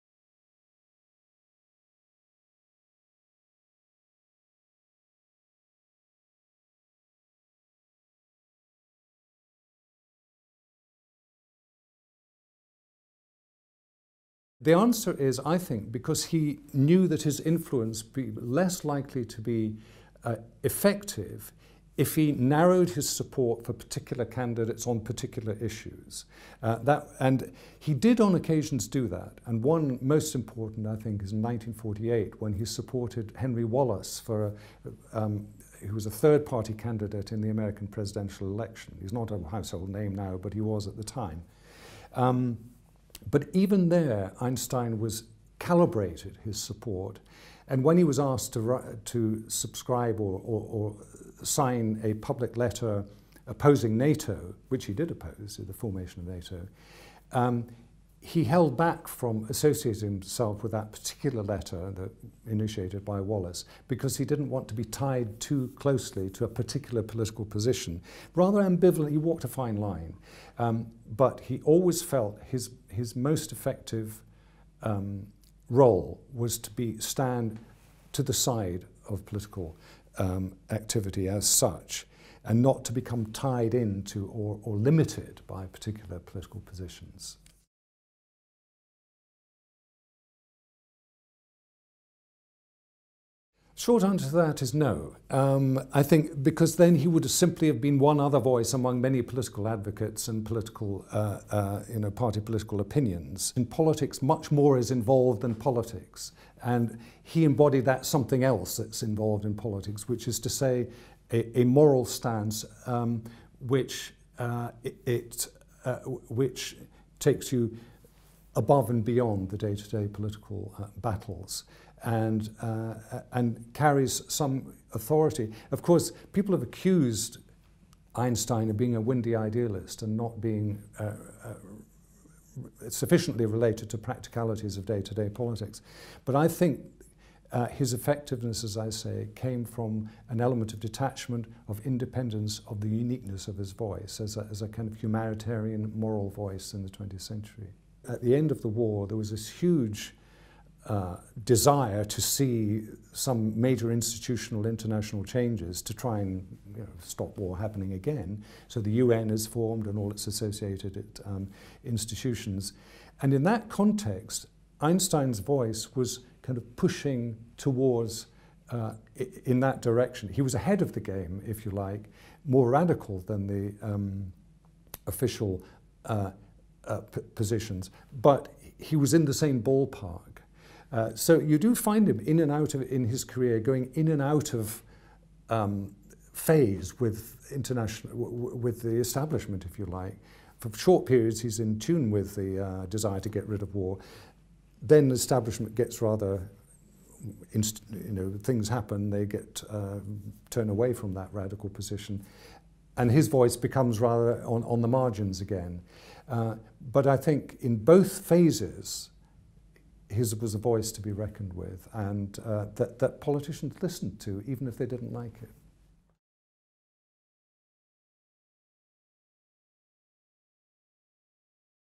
Did Einstein have political influence?
Richard Crockatt, author of Einstein and Twentieth-Century Politics 'A Salutary Moral Influence', looks at why Einstein did not often voice his political position and whether he could be considered to have influenced politics.

Richard Crockatt, Emeritus Professor of American History, University of East Anglia. Born in Scotland in 1947, Richard Crockatt studied English Language and Literature at Edinburgh University. After two years in Germany he studied for a D.Phil at Sussex University on the topic of American liberalism. In 1978 he was appointed lecturer in American History at the University of East Anglia from which he retired as professor in 2011. Specialising in American foreign policy and international relations in the twentieth century, he is author of The Fifty Years War: The United States and the Soviet Union in World Politics, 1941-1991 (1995), America Embattled: September 11, Anti-Americanism, and the Global Order (2003), and After 9/11: Cultural Dimensions of American Global Power (2007). He has also published throughout his career in the field of intellectual history.